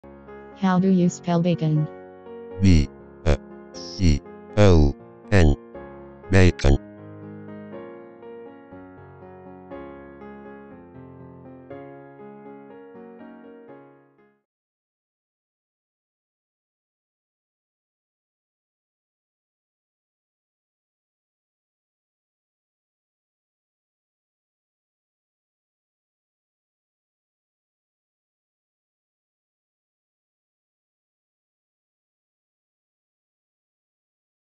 Learn how to spell bacon
Learn how to spell and pronounce bacon.